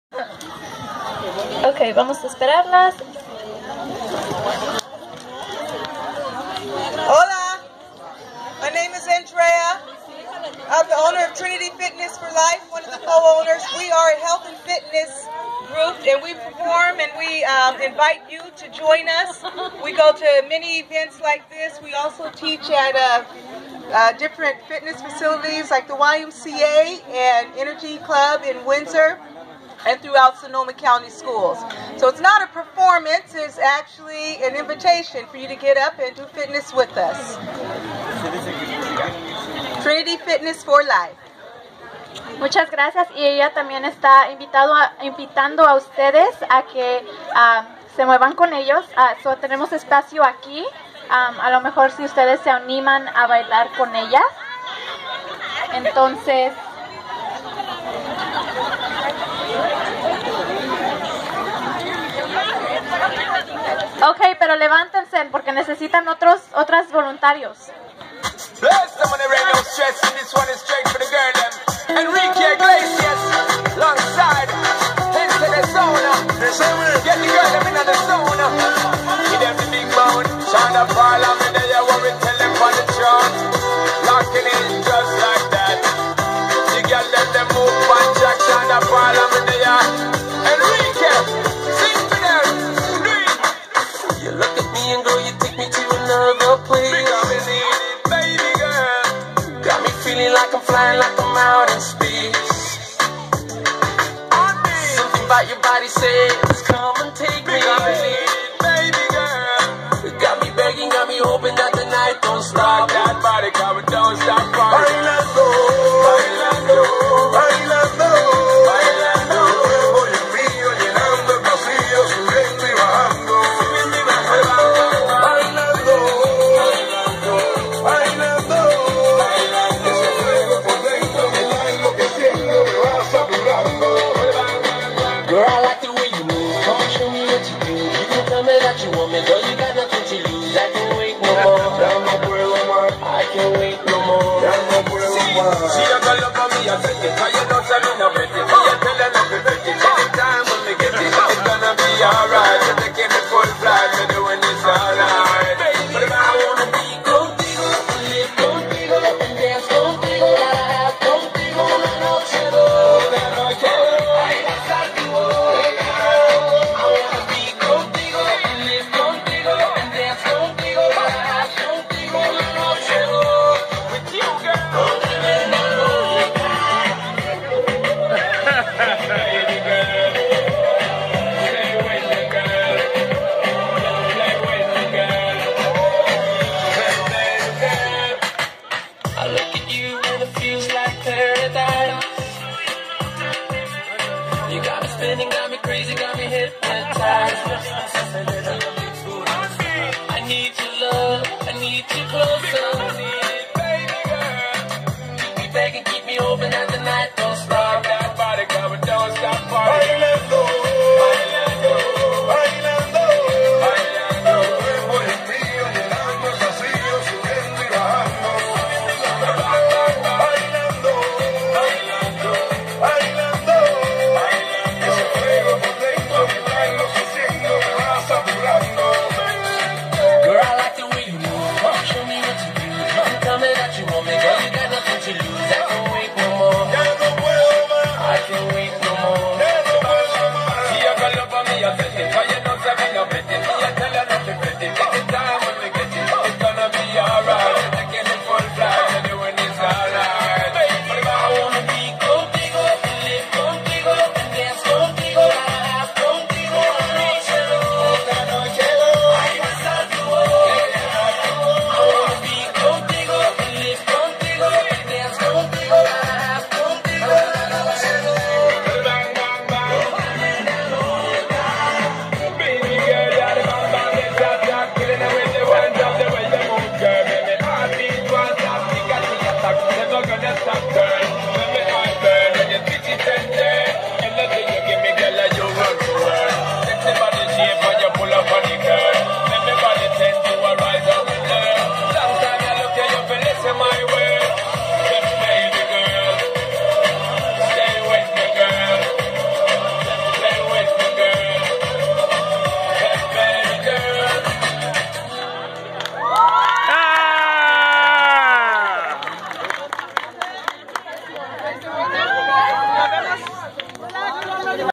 Fitness in the Park with Trinity Fitness 4 Life
ZUMBA!!! Wholly Power Productions Presents Trinity Fitness 4 Life at the Annual South Park Day and Night Festival!